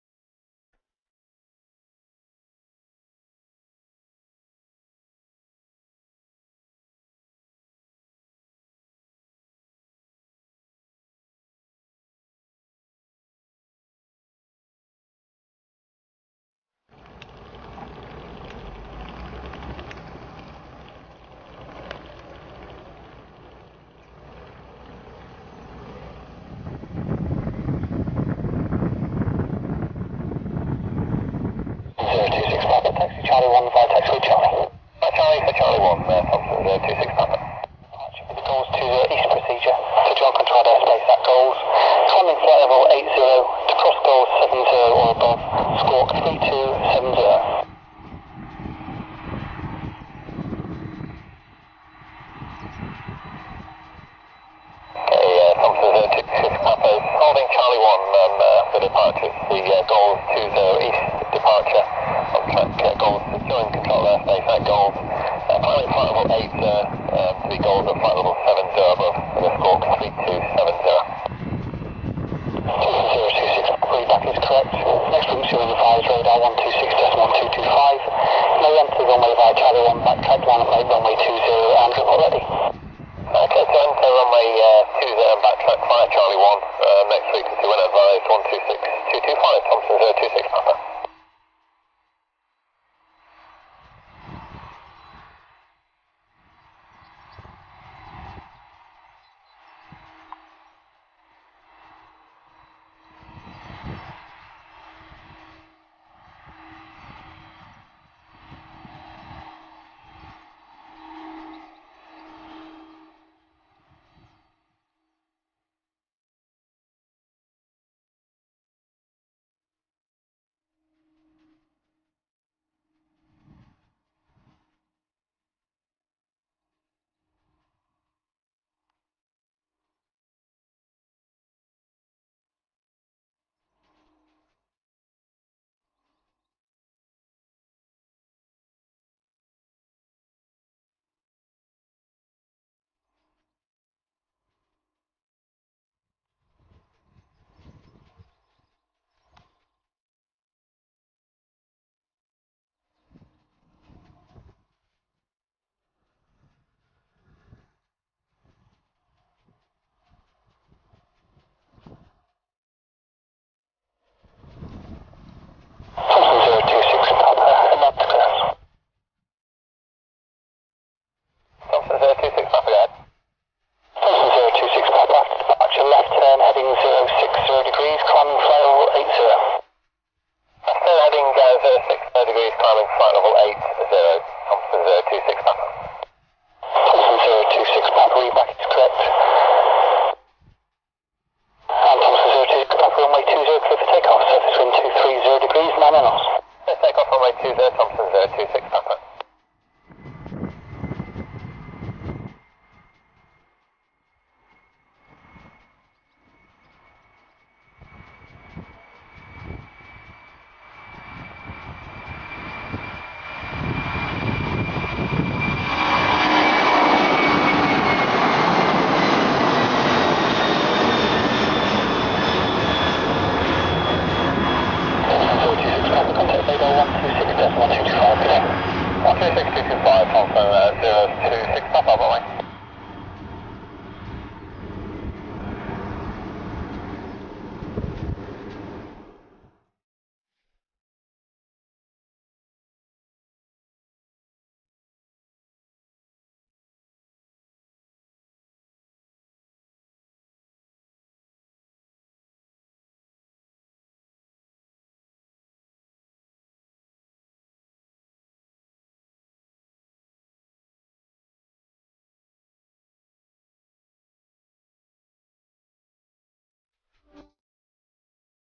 B767-204/ER G-BYAA FOR MANCHESTER @ DONCASTER AIRPORT (UK)
No119 VIDEO OF THOMSON B767-204/ER,
G-BYAA, TOM026 PAPA DEPARTING RUNWAY TWO ZERO FOR MANCHESTER,
BY M CONNOLLY MONDAY 14th JULY 2008.